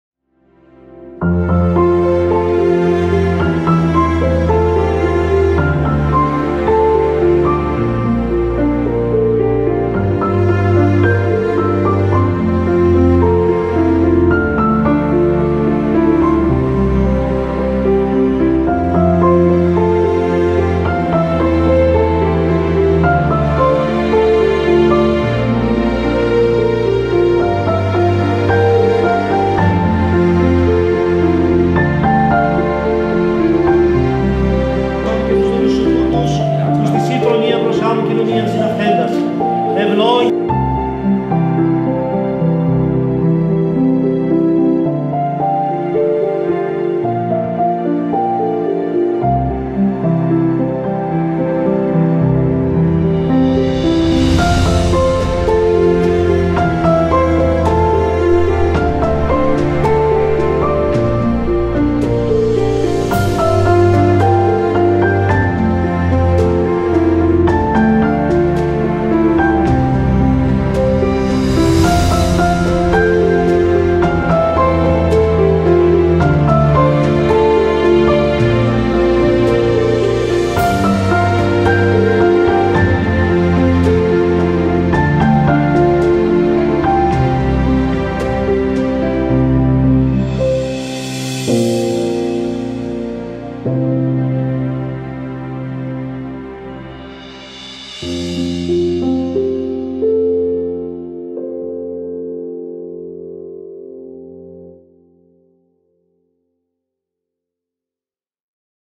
Νικόλας & Μαρία - Trailer 2021
Responsible for Wedding Cover - Phivos Pentes
Videography - Petros Sochos
Editing - Evans Panag

A Trailer Shot at Patmos Island during May 2021.